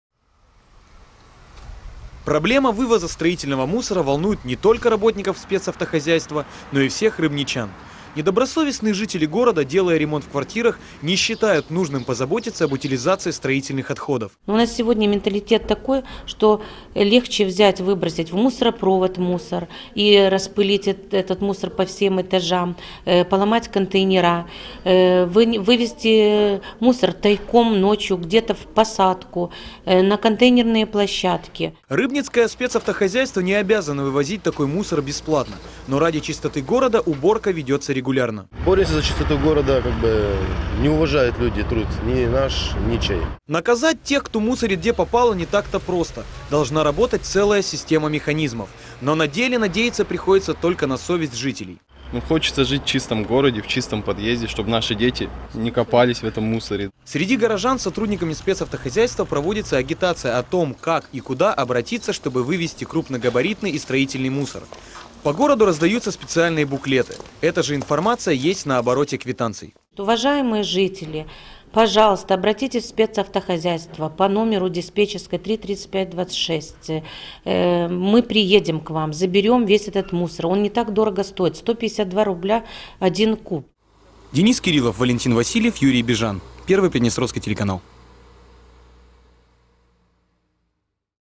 Проблема вывоза строительного мусора волнует не только работников «Спецавтохозяйства»
В Рыбнице ежедневно коммунальщики вывозят тонны мусора. Работа обычная - повседневная, но прибавляются и трудности. Проблема вывоза строительного мусора волнует не только работников «Спецавтохозяйства», но и всех рыбничан. Недобросовестные жители города, делая ремонт в квартирах, не считают нужным позаботиться об утилизации строительных отходов.